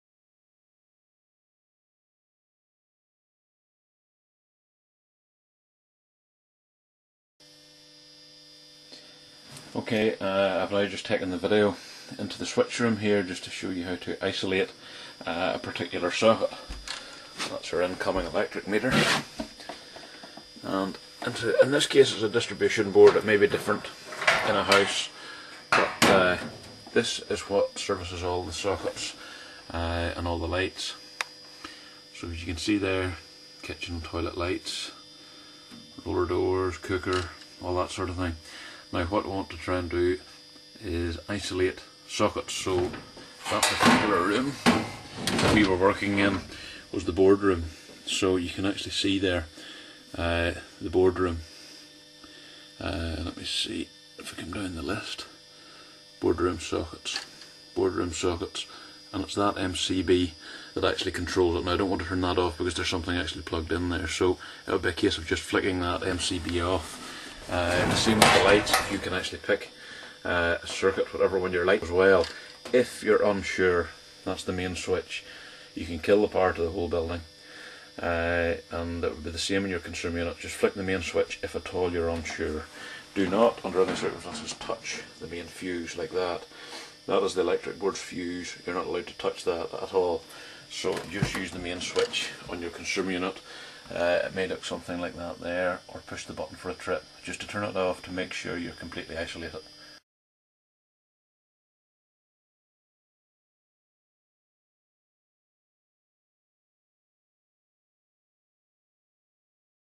How To Make A Circuit Safe Turn off Electricity Safely
Our new website shows how hundreds of other customers have slashed their electricity bill.

In this video we show how to isolate a circuit to make it safe to work on.